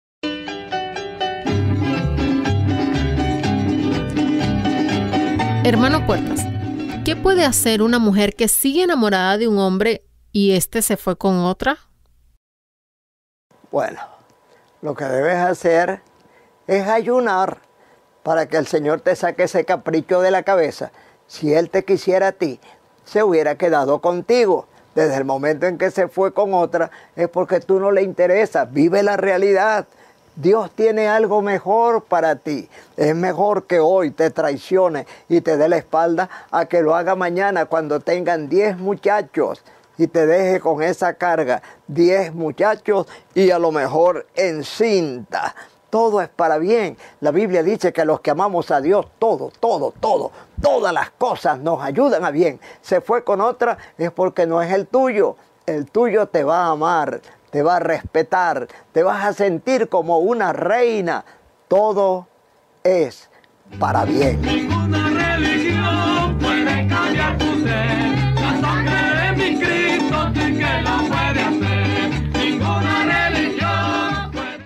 HNO PUERTAS RESPONDE #311 | ¿QUÉ HAGO ME ENAMORO Y SE FUE CON OTRA? |LUZ TV | MICRO HNO PUERTAS
Hno. Puertas; ¿QUÉ PUEDE HACER UNA MUJER QUE SIGUE ENAMORADO DE UN HOMBRE Y ESTE SE FUE CON OTRA?

Voz Over / Locutor: que lee las preguntas:
Juan Marquez.